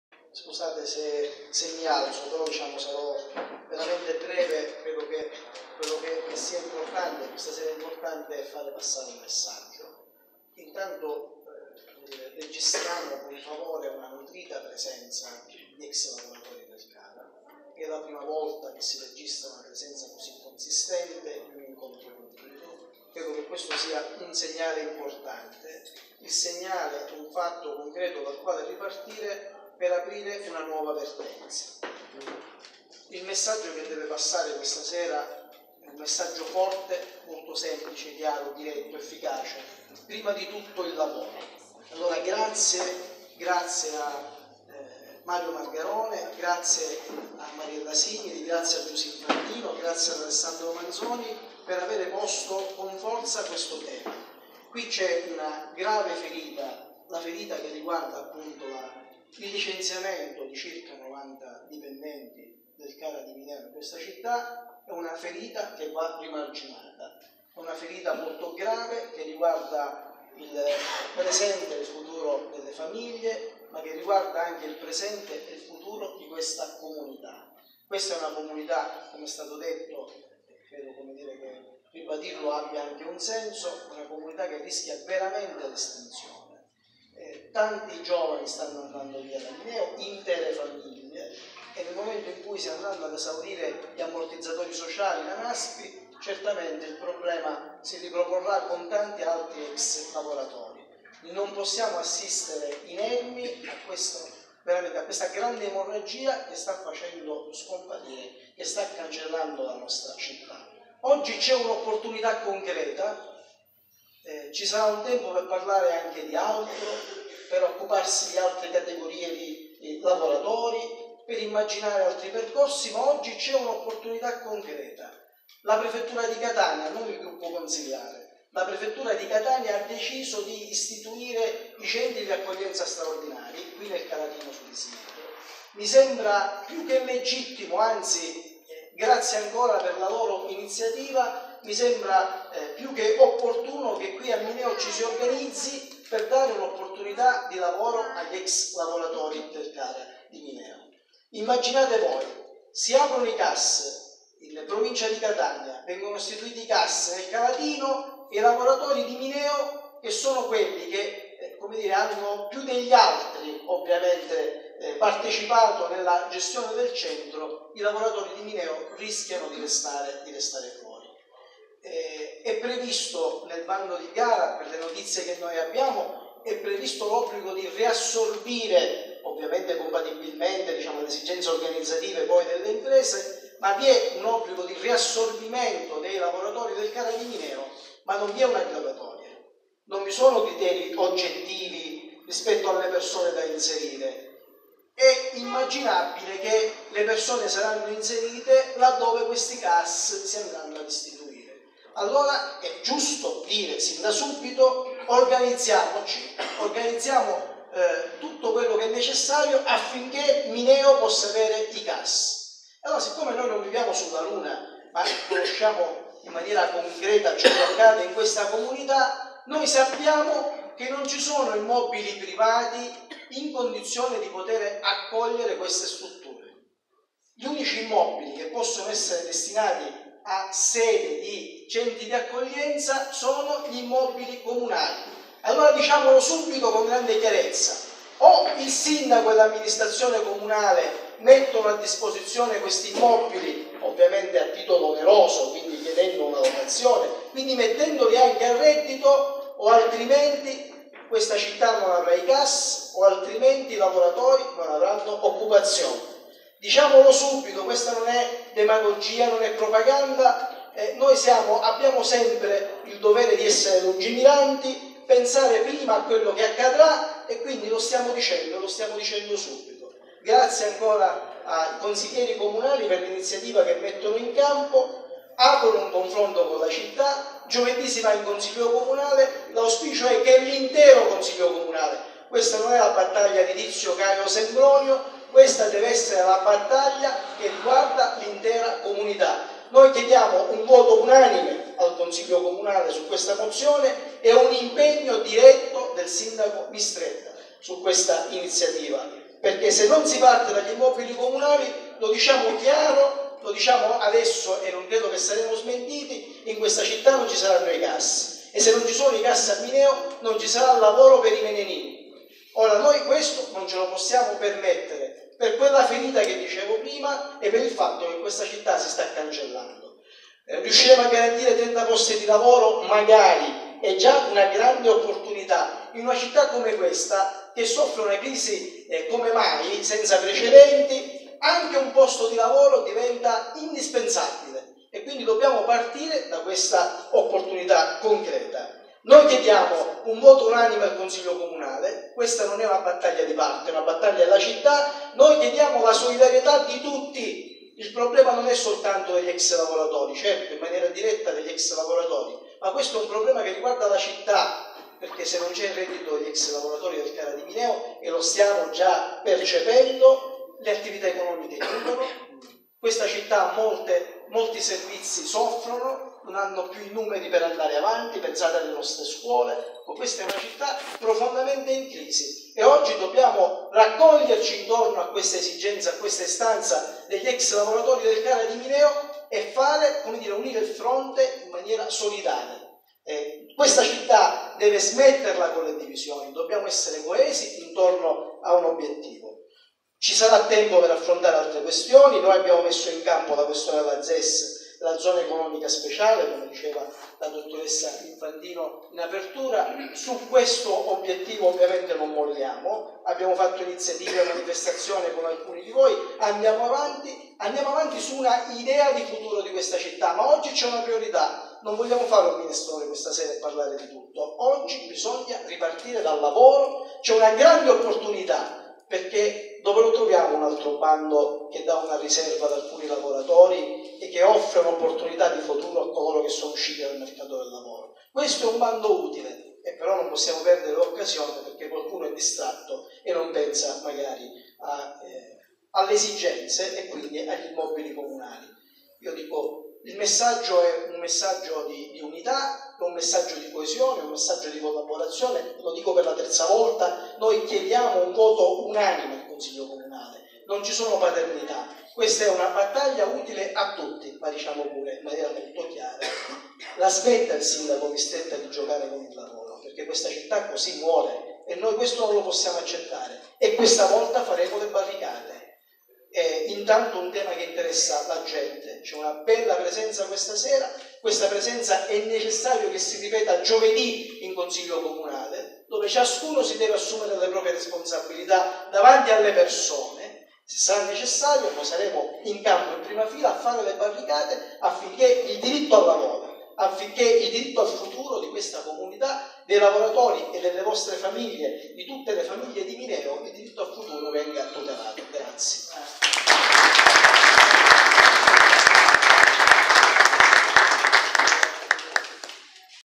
Intervento di Paolo Ragusa sull'isituzione dei CAS a Mineo
La mozione è stata presentata dal Gruppo Consiliare CambiAmo Mineo